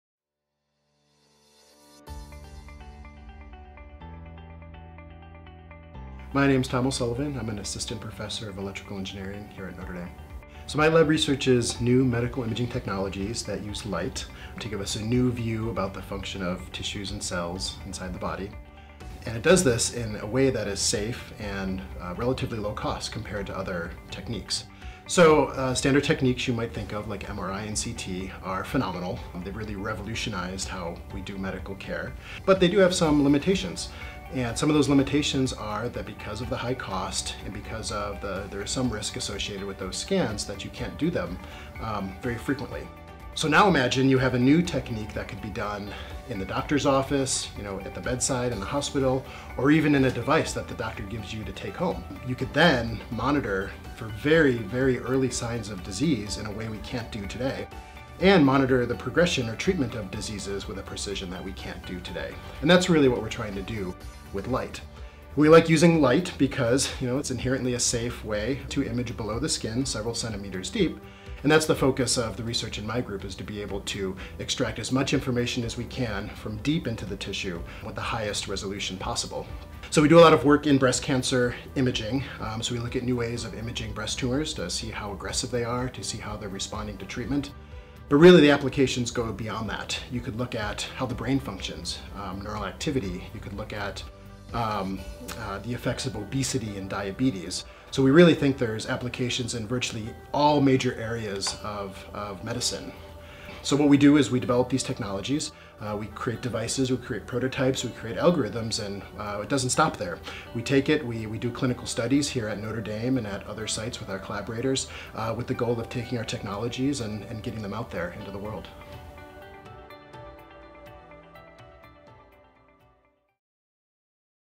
Researching to Heal: Developing Better Tools to Fight Cancer
Prof. Thomas O'Sullivan, an assistant professor in the Department of Electrical Engineering at the University of Notre Dame, shares about the work of his research group. His team is taking strides to improve medical care, particularly cancer care, with their advanced imaging technologies. Utilizing near-infrared light (more specifically, diffuse optics), they develop technologies to safely and non-invasively image tissue deep beneath the skin. This work, while applicable to many diseases, is presently focused on breast cancer for risk assessment, screening, differential diagnosis, and predicting individual responses to treatment.